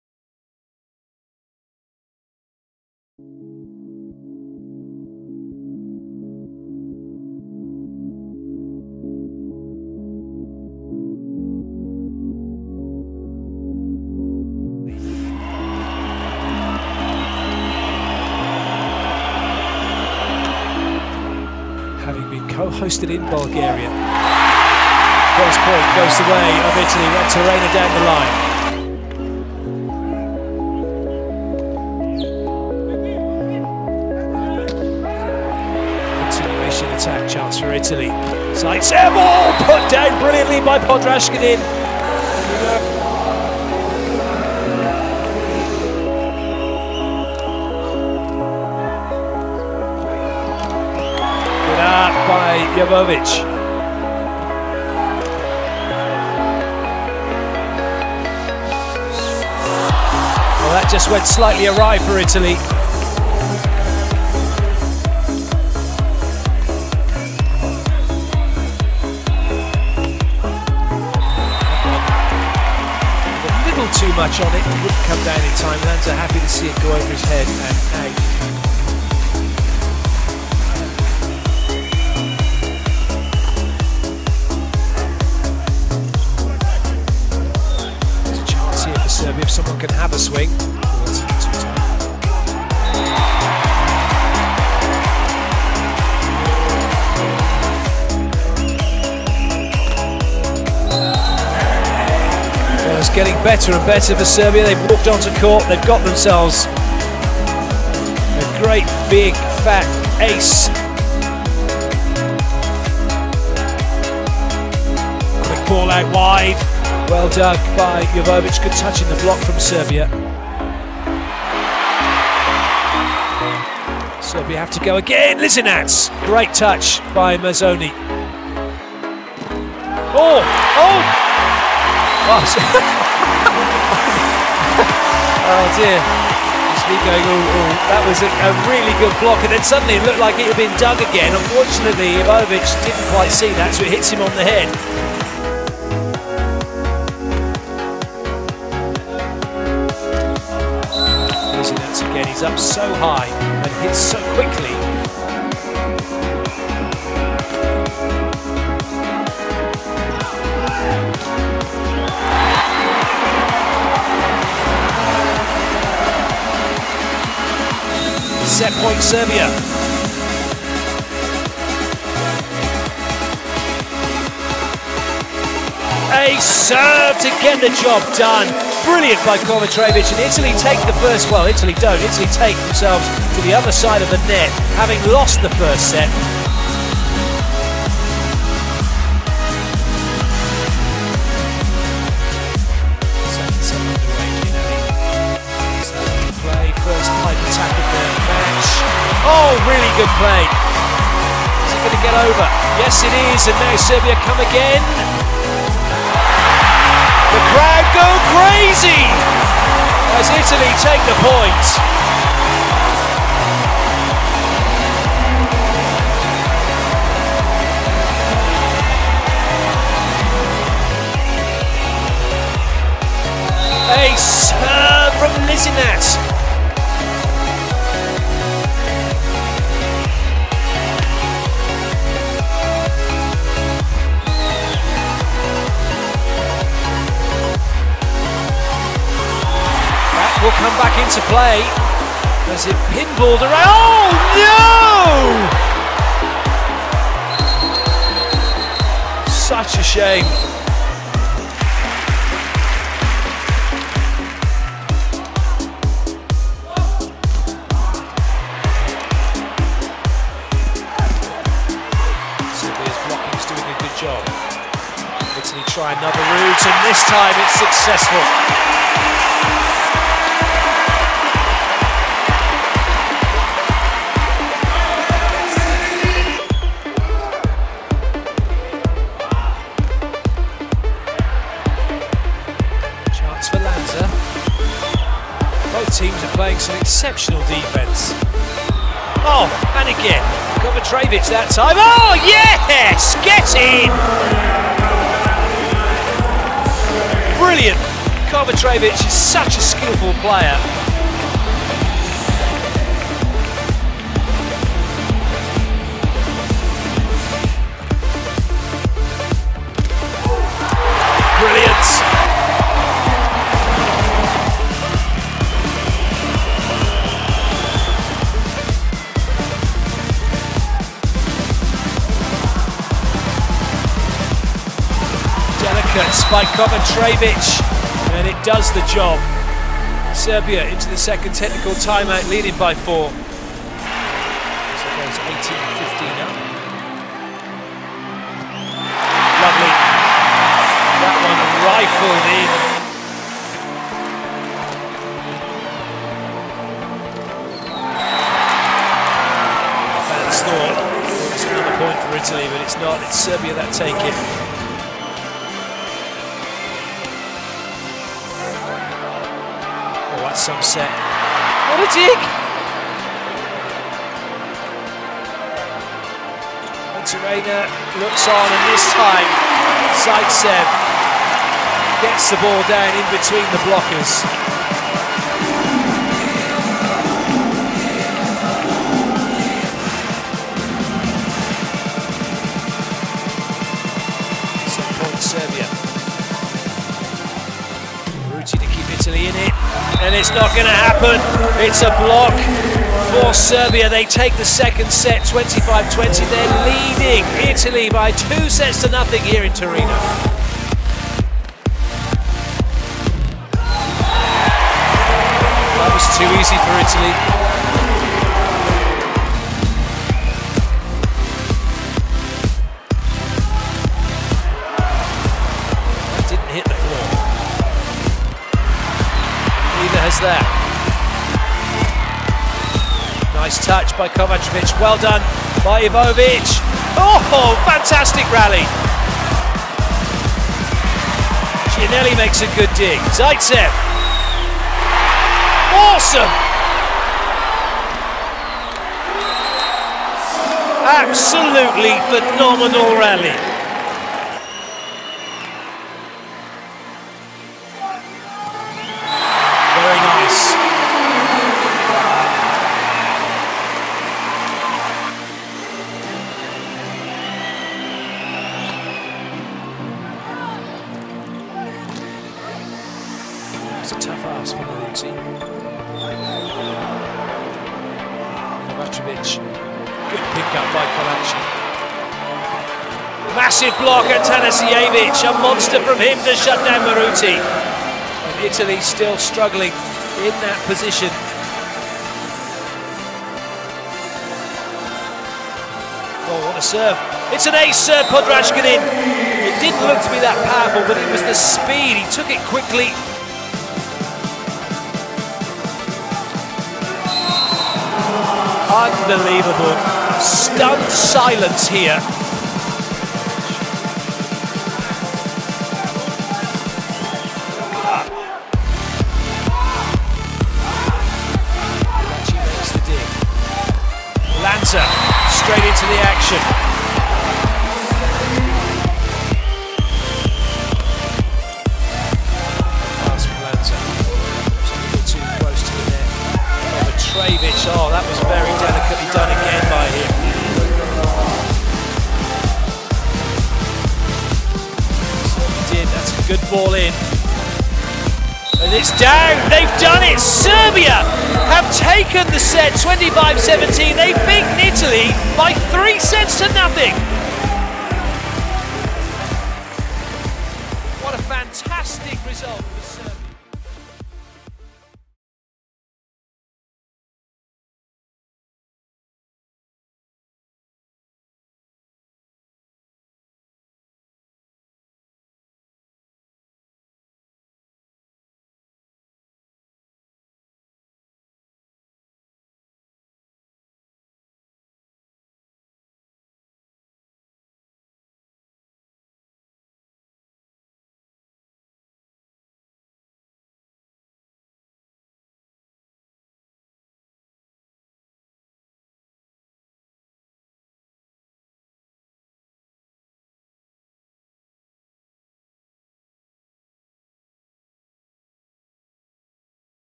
Italy vs. Serbia | Highlights | Final 6 Mens World Championship 2018
Turin, Italy, September 26, 2018 - A shocking start for Italy as the hosts of the 2018 FIVB Men's World Championship Final Six were hammered by the veteran outside hitter of Serbia Marko Ivovic in a clear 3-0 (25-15, 25-20, 25-18), despite 11,700 warm and enthusiastic fans on their side.

Some 11,700 people supporting you can be a bit too much to handle, especially in the beginning: it might take a while before you can collect all their energy and make it yours.

Italy just couldn't, in their first night as hosts of the Final Six in Turin, against too much Serbia.

Ivovic's serve was the spring to every Serbian effort: impressive for power and accuracy (4 aces, 5 errors).

Italy, on the other hand, could not repeat their formidable offensive performances: the outside hitters were encaged in Nikola Grbic's strategy (both Filippo Lanza and Osmany Juantorena below 40% efficiency), and home hero Ivan Zaytsev encountered strong opposition (29% attack and only 9 points in the end).

Italy eventually gained some momentum early in the third set thanks to killer blocks: Daniele Mazzone (5) provided plenty